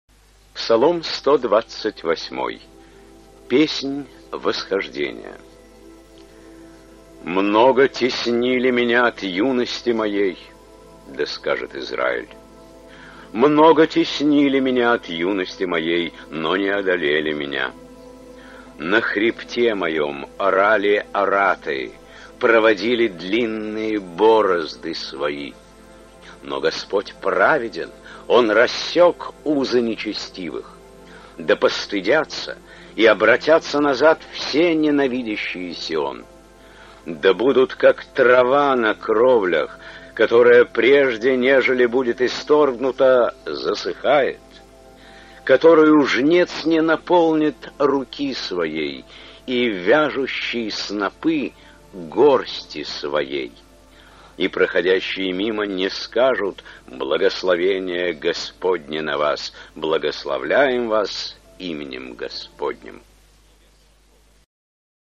Читаем Библию Вместе 18.10.2024.Псалом 128
1 Много теснили меня враги, но Господь рассек узы нечестивых и спас меня. 5 Да будут постыжены и теперь все враги мои, да обратятся они в засохшую траву и пусть не услышат благожелания успеху своего дела.